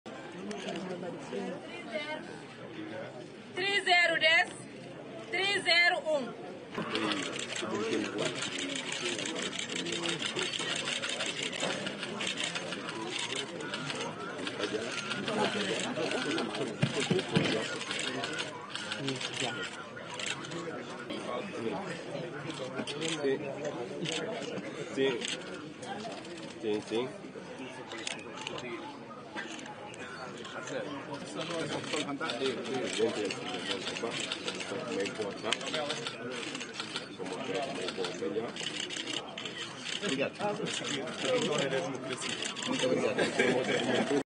Le président de Guinée-Bissau vote pour les législatives
Le président de la Guinée-Bissau José Mario Vaz vote pour les élections législatives. Quelque 760.000 inscrits sont appelés aux urnes de 07H00 à 19H00 (GMT et locales) pour un scrutin à la proportionnelle à un tour, avec pour la première fois un minimum de 36% de femmes candidates sur les listes des 21 partis en lice, pour 102 sièges à pourvoir. Les premiers résultats sont attendus 48 heures plus tard. IMAGES